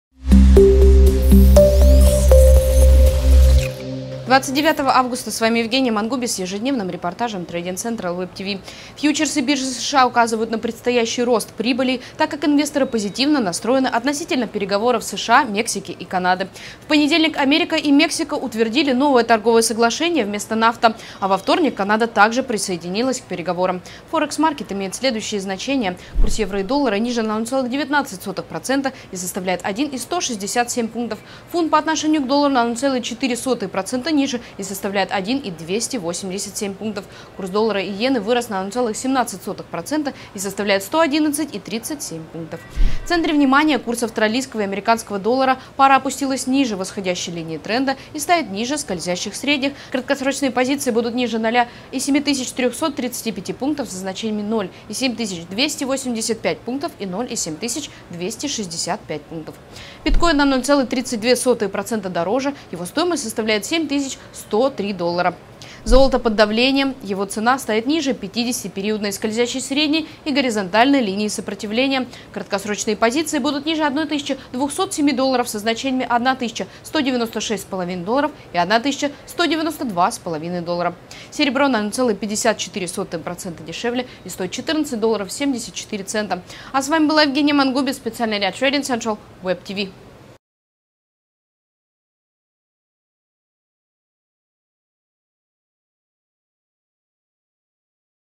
Фьючерсы биржи США указывают на рост котировок
08/29/18 Фьючерсы биржи США указывают на предстоящий рост прибылей, так как инвесторы позитивно настроены относительно переговоров США, Мексики и Канады.